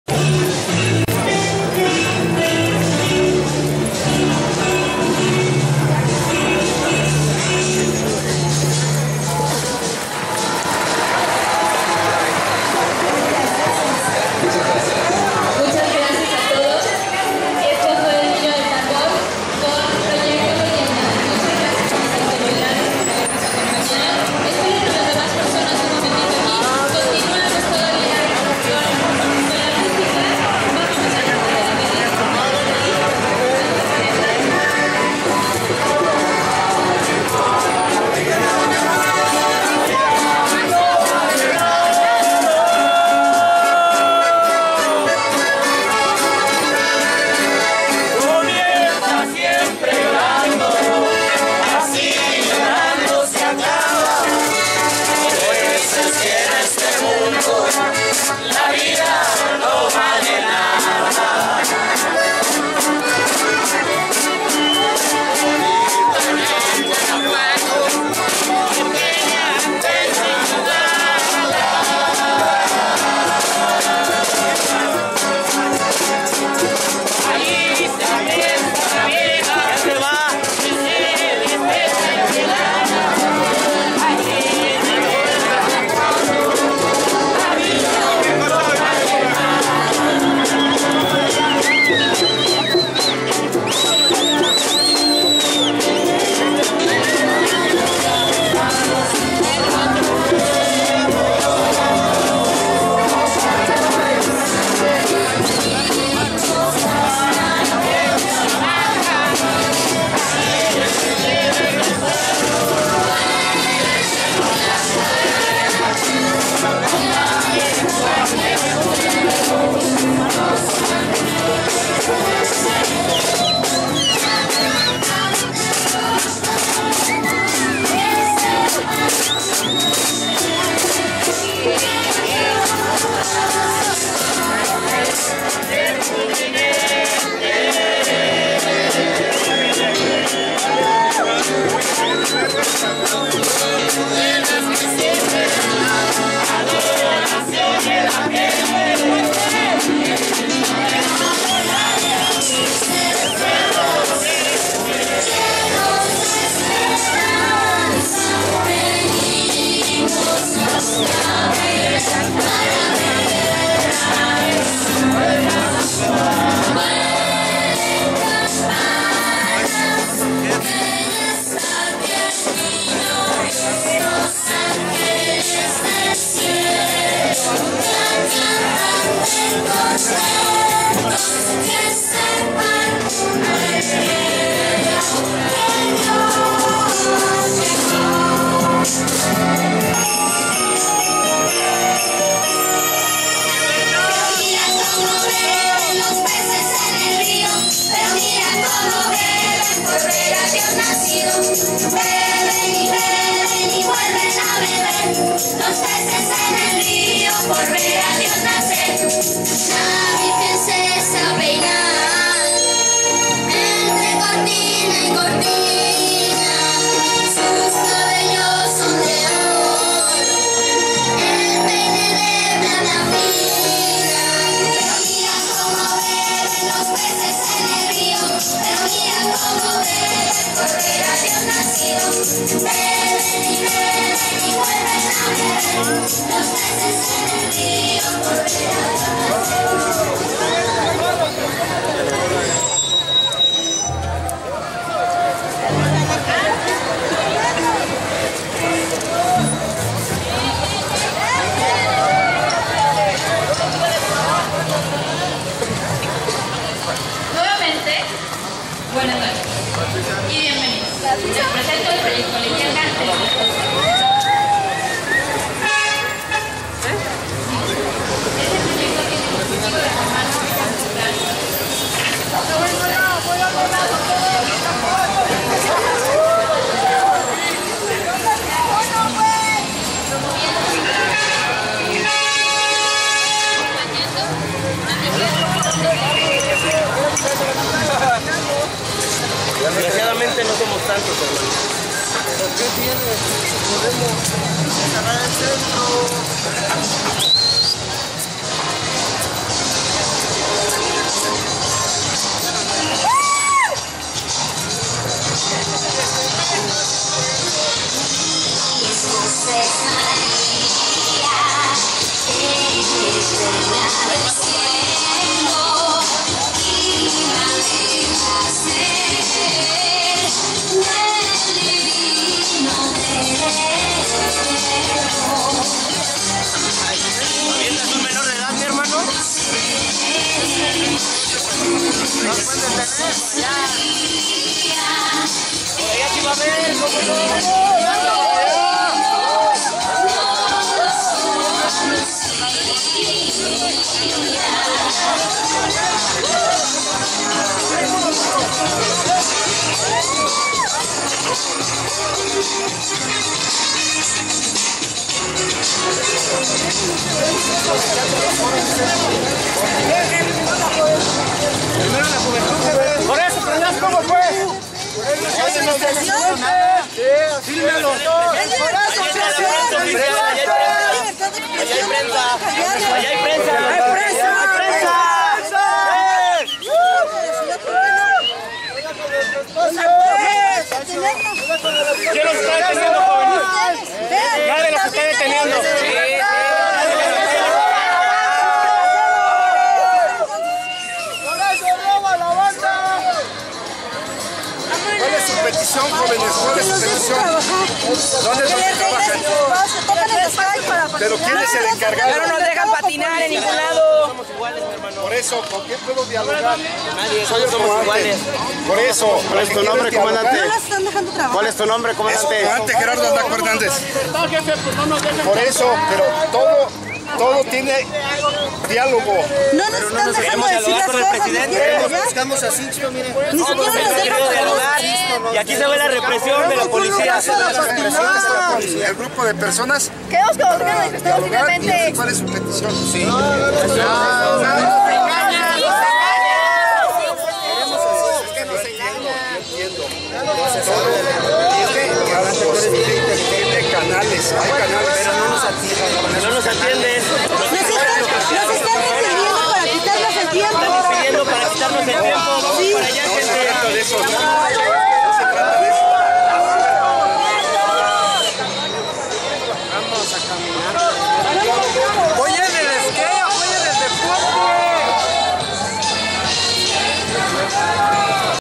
REPRESION...SIXTO ALFONSO ZETINA PRESIDENTE MUNICIPAL DE IRAPUATO NO LE IMPORTAN LOS JOVENES...
UN GRUPO DE JOVENES ENTRE MUSICOS, ESCATOS Y BAILARINES SE MANIFESTARON DURANTE LA INAUGURACION DE LA VILLA NAVIDEÑA DE IRAPUATO, EN LA PLAZA DEL AGORA, LA POLICIA SE DEDICO A AGREDIRLOS MAS QUE A PERSUADIRLOS POR ORDENES DE LA SUPREMA AUTORIDAD...